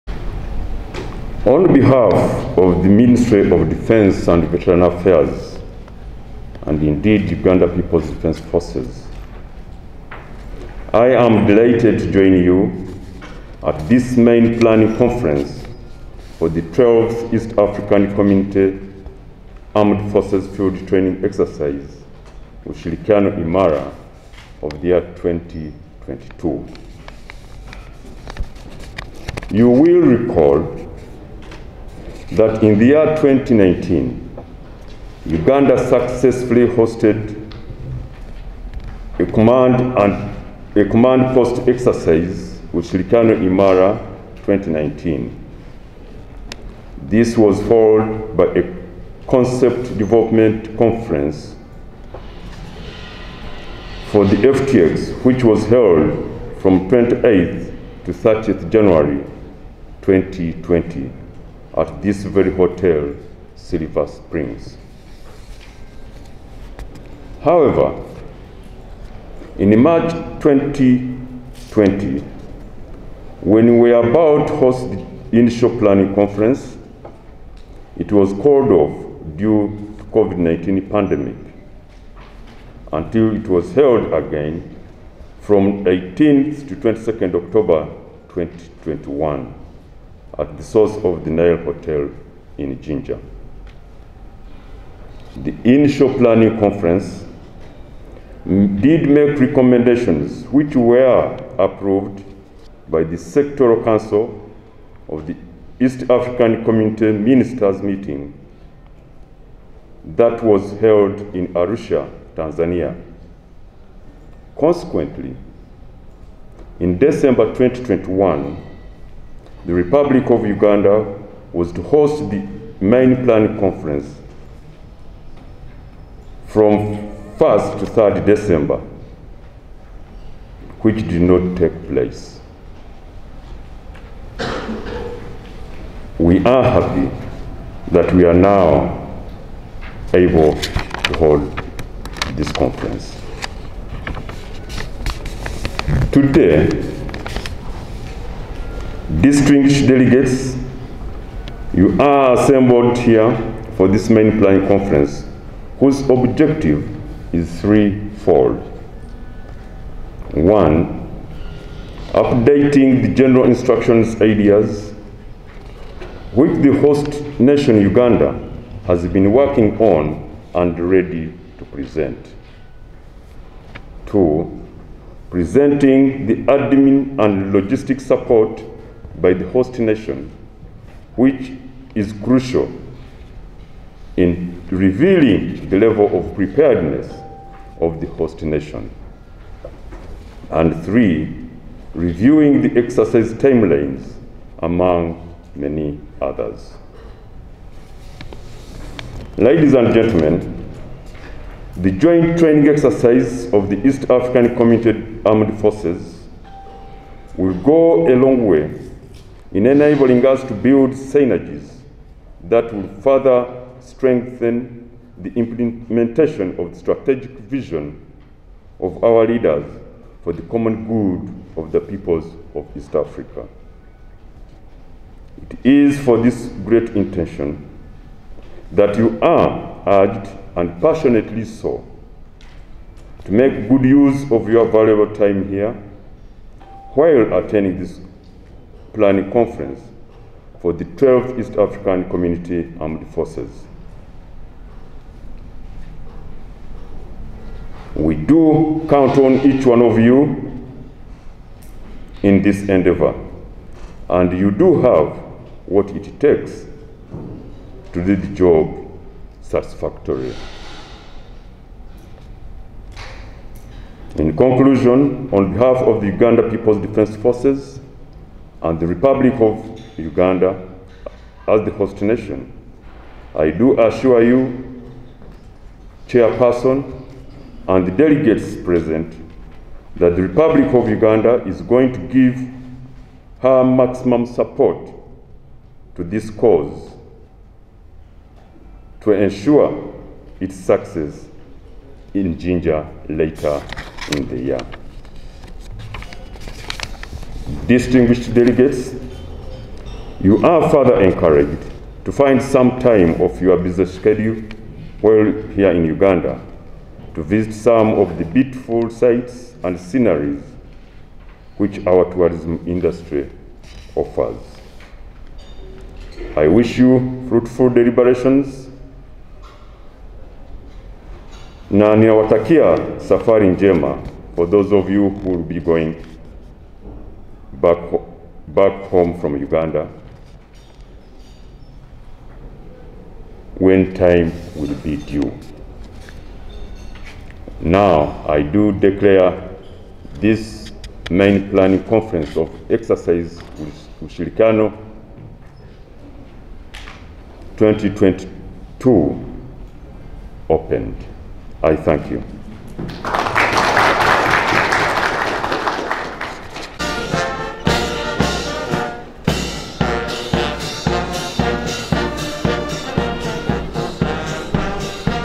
UPDF MAJ GEN GEORGE IGUMBA BRIEFING THE EAST AFRICA ARMED FORCES
UPDF MAJ GEN GEORGE IGUMBA BRIEFING THE EAST AFRICA ARMED FORCES PLANING FIELD EXERCISE AT SILVER SPRINGS BUGOLOBI